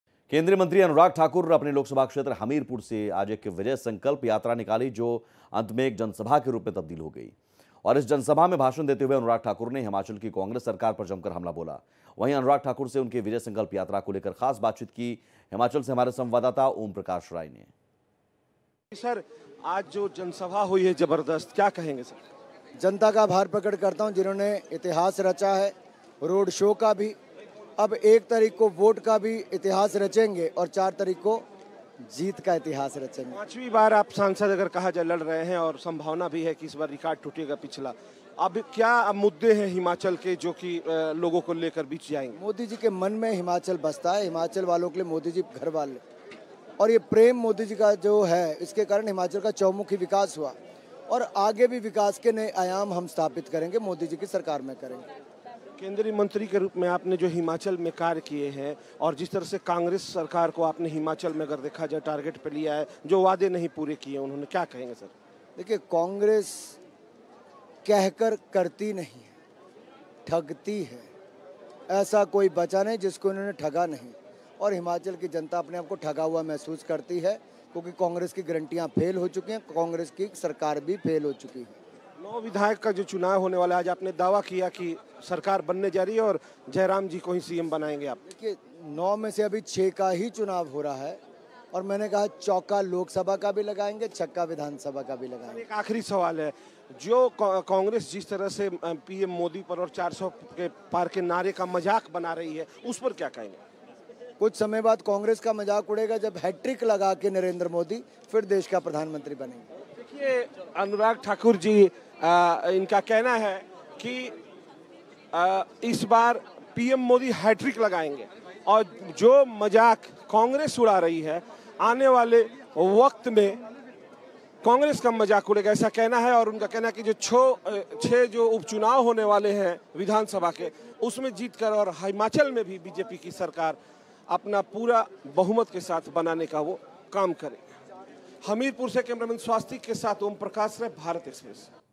Lok Sabha Election: Bharat Express पर बोले केंद्रीय मंत्री Anurag Thakur, "कांग्रेस कह कर करती नही"
Thanks & Regards
Bharat Express